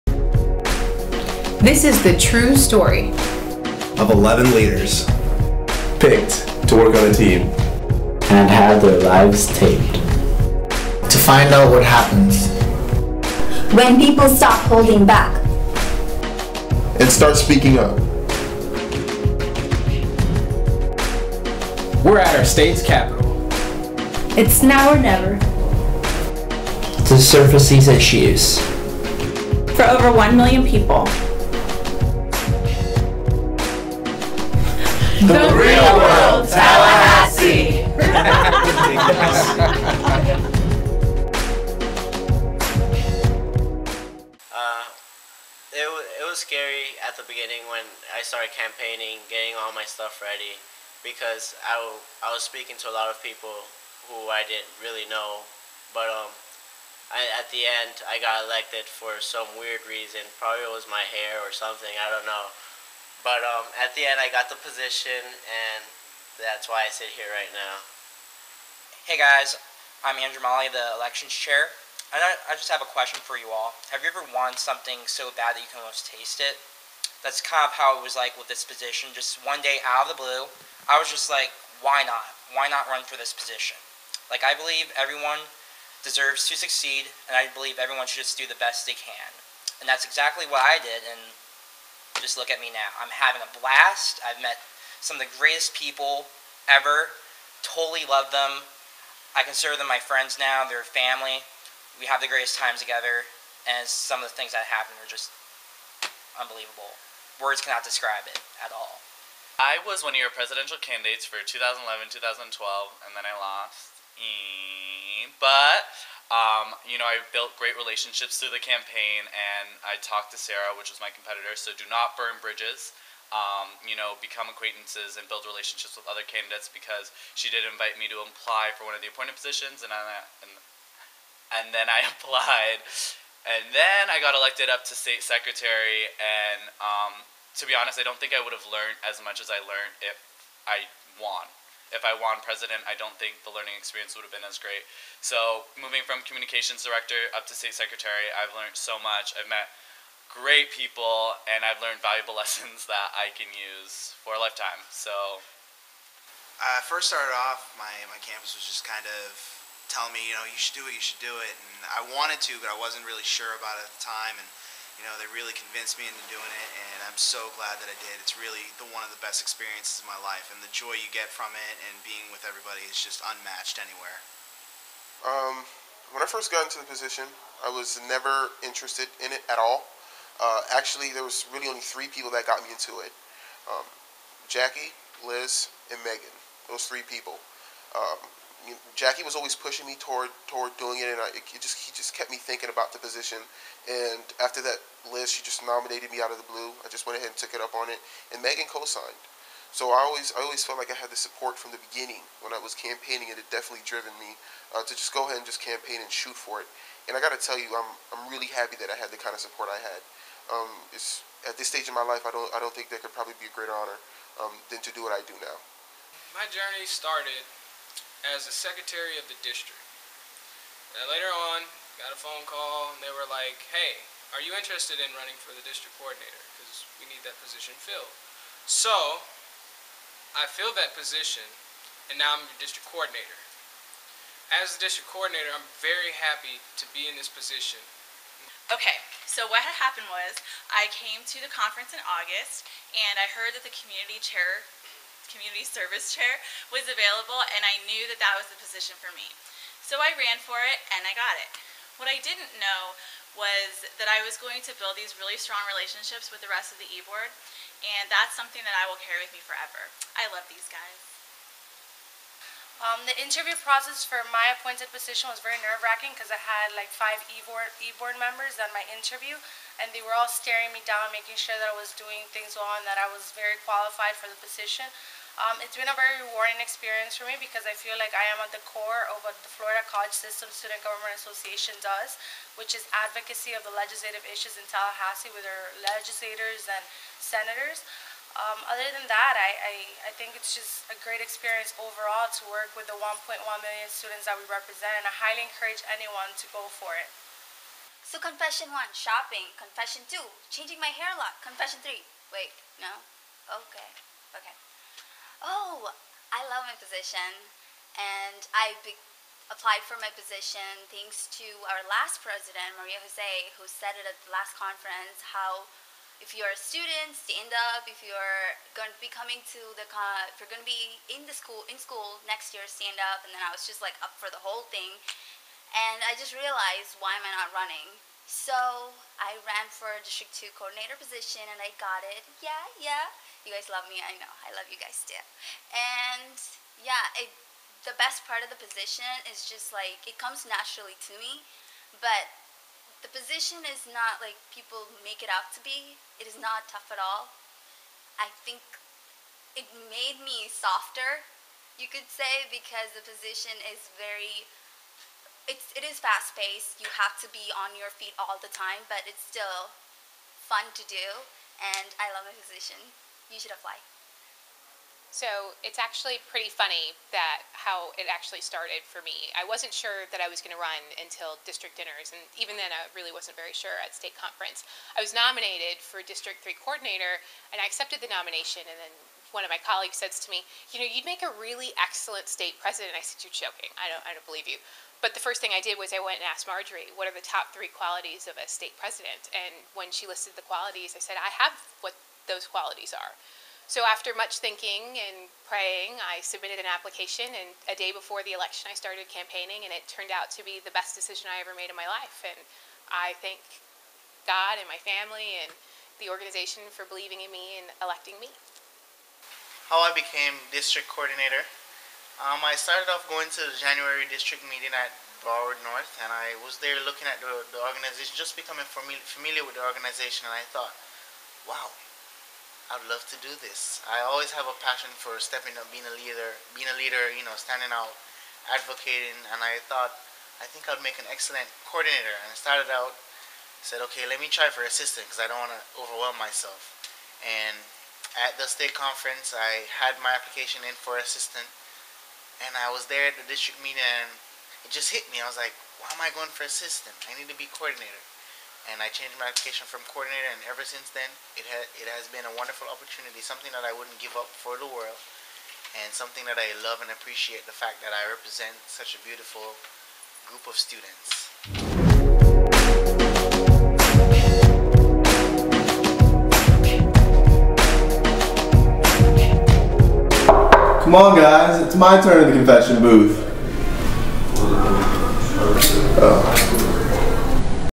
Real World Tallahassee (Tallahassee Conference 2012)
The E-Board says their story on how they got to be where they are today. Funny, sad, and serious, all E-Board members are included in this video including the newest member Alistair who wanted his time in the "confession booth".

[Imitation of MTV's Real World]